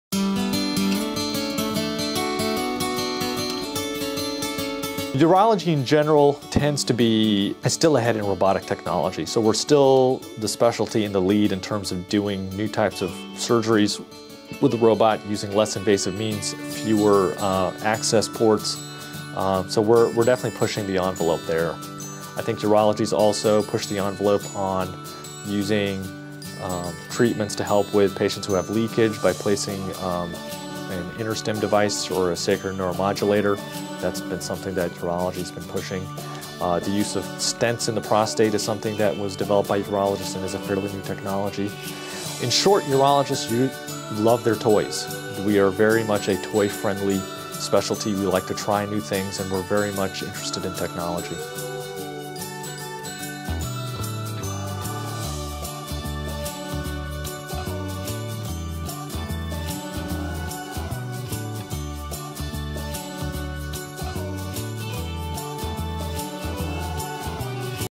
Cutting Edge Technology Used In Urology - SLUCare Urology
Dr. Sameer Siddiqui talks about the advancement of technology in urological surgical procedures including robotics, stents and more.